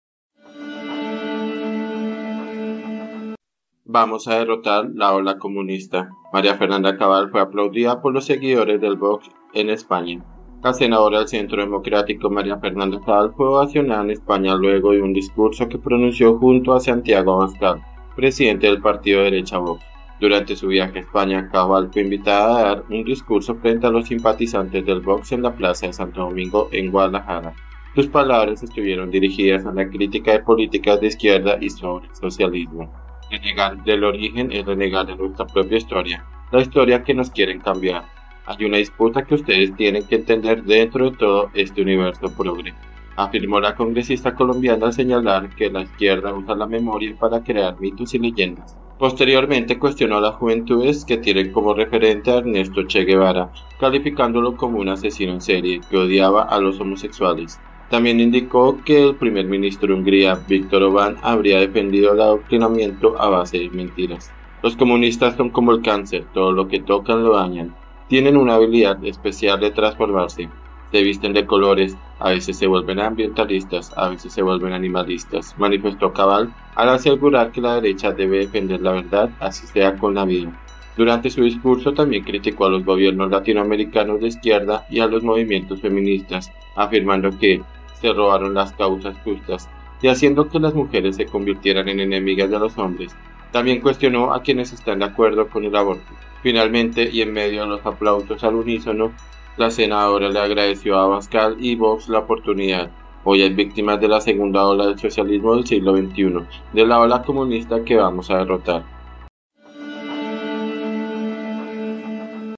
"VAMOS A DERROTAR LA OLA COMUNISTA" ▶️ Cabal Aplaudida en España
Durante su viaje a España, Cabal fue invitada a dar un discurso frente a los simpatizantes del Vox en la Plaza de Santo Domingo en Guadalajara. Sus palabras estuvieron dirigidas a la crítica de políticas de izquierda y sobre el socialismo.